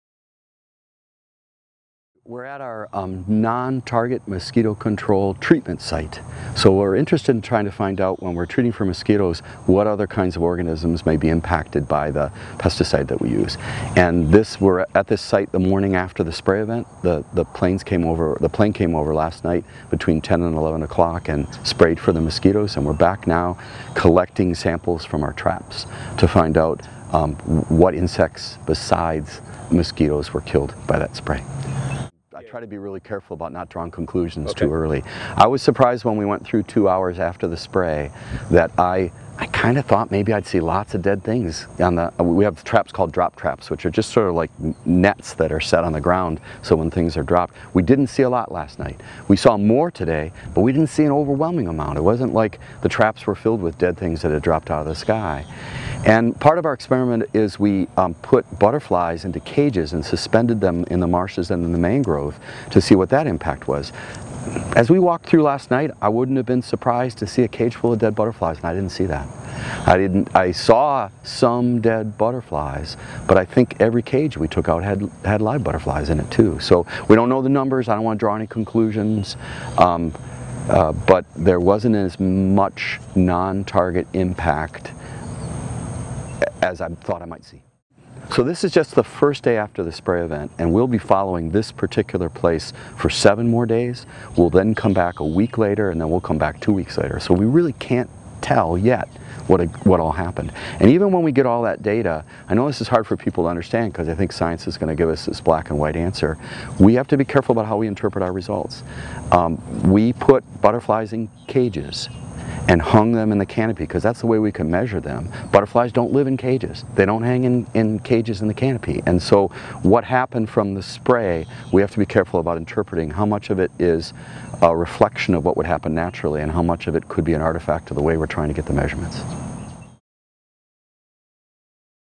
FGCU Biologists and Lee County Mosquito control study the effect of spray on non target species
FGCU Biologists and Lee County Mosquito control study the effect of spray on non target species.  (Video by Andrew West/news-press)